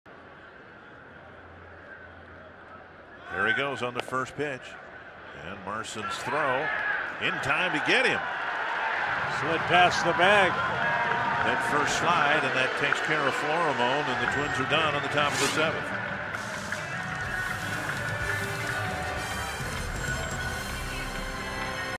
2012/09/20 Marson nabs Florimon
MIN@CLE: Marson fires to catch Florimon stealing